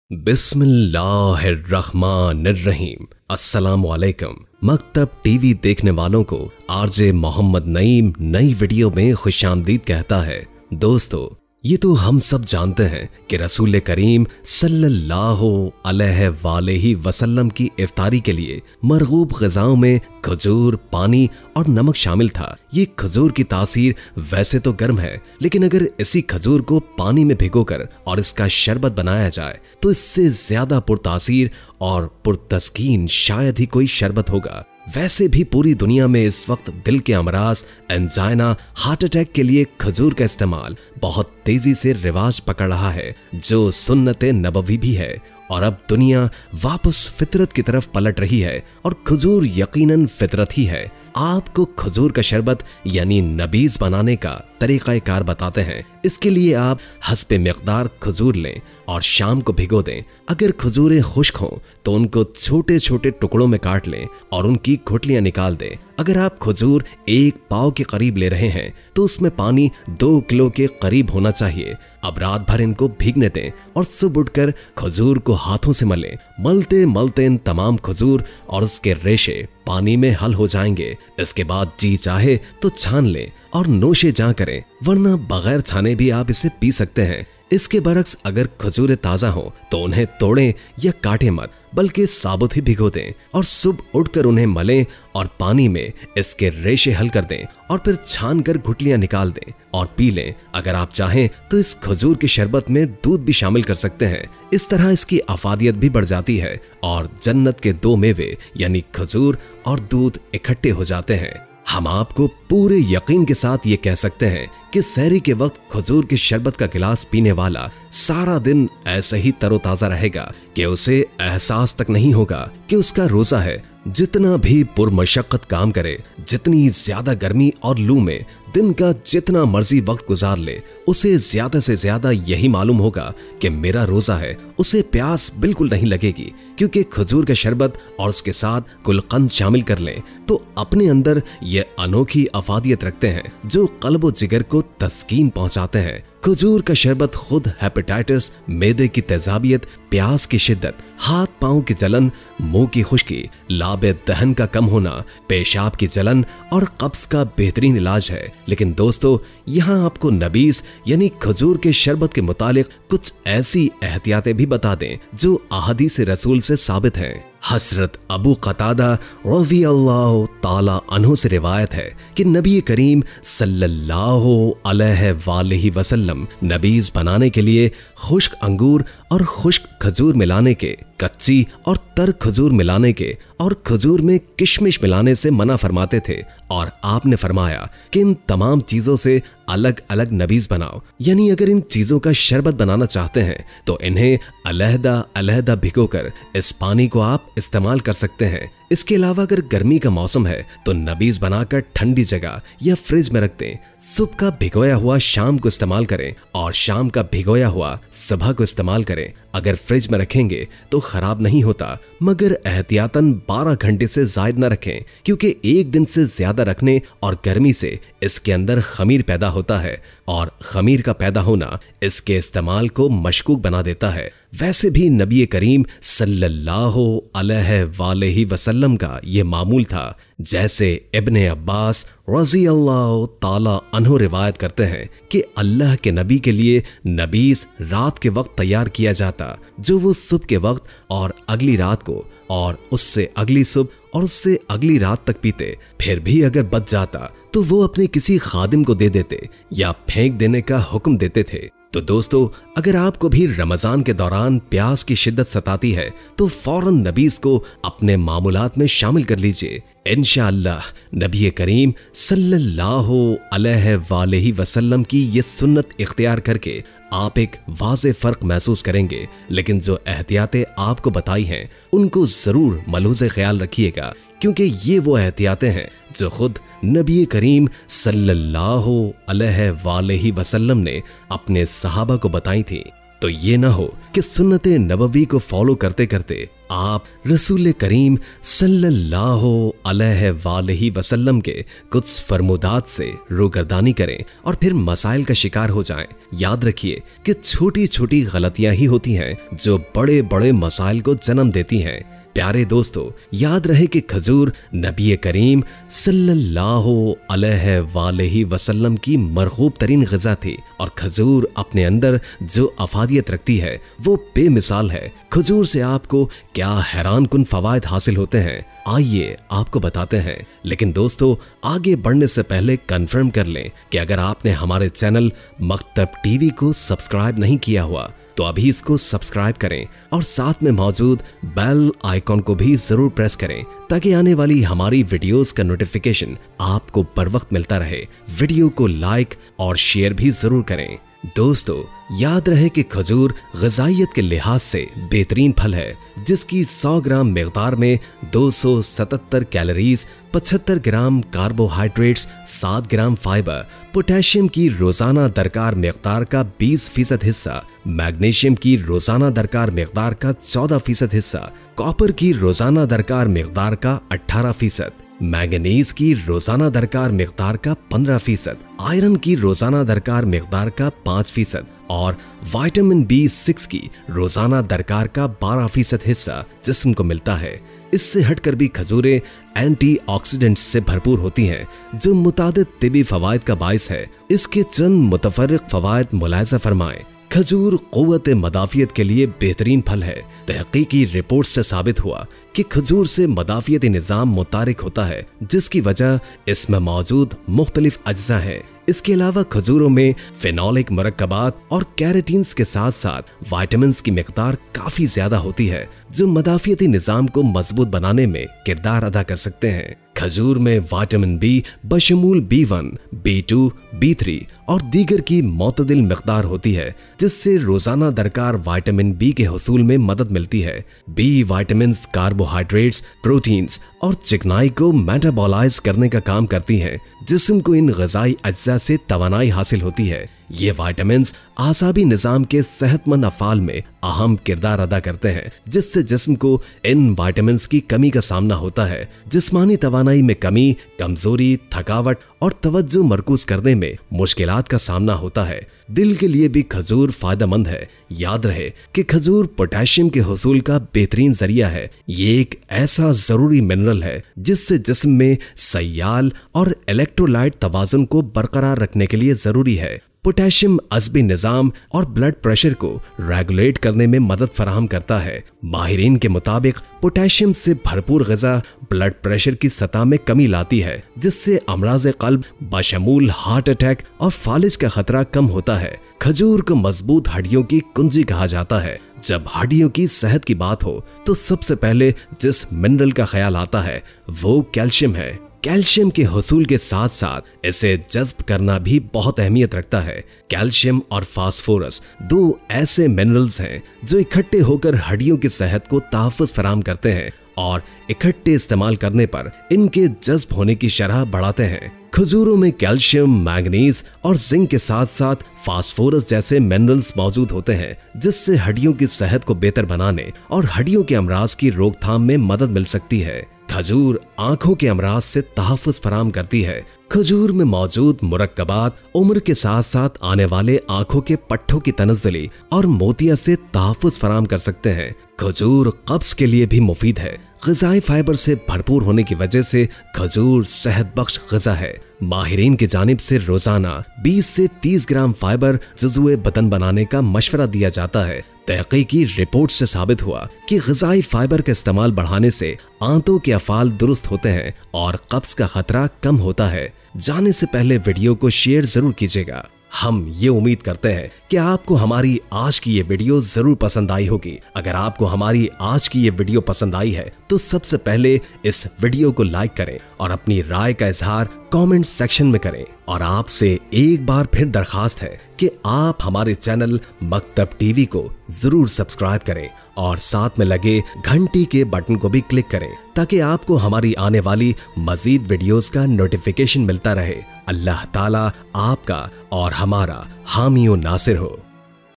Ramzan Energy Drink | Favourite Drink of Prophet Muhammad PBUH | Maktab TV
دوستو آج کی اس ویڈیو میں آپ کو حضرت محمدﷺ کے پسندیدہ شربت کے بارے میں بتائیں گے اور ساتھ ہی ساتھ آپ کو اس شربت کے فوائد کے بارے میں بتائیں گے آپﷺ نے اس شربت کے براے میں کیا ارشاد فرمایا اور یہ شربت کیسے بنتا یے یہ سب جاننے کے لیے اس ویڈیو کو آخر تک لازمی دیکھے اور اس شربت کو افطاری میں استعمال کریں اور نبی پاکﷺ کی سنت کو ادا کرنے اور اس سنت کو دوستوں کے ساتھ بھی شیئر کریں